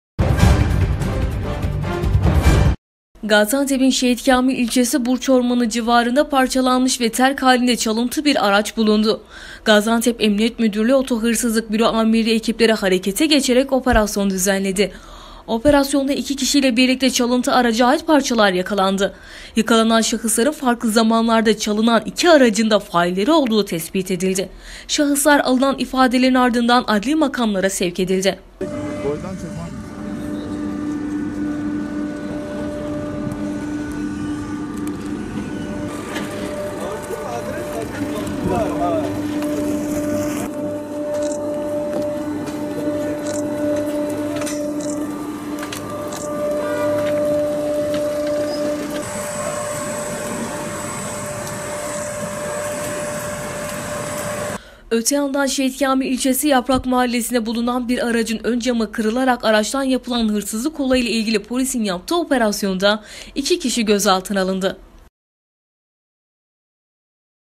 ÇALINTI ARAÇ PARÇALANMIŞ HALDE BULUNDU
Çalınan bir araç ormanlık alanda parçalanmış halde bulundu. Polis olayla ilgili yaptığı operasyonda 3 kişiyi gözaltına aldı.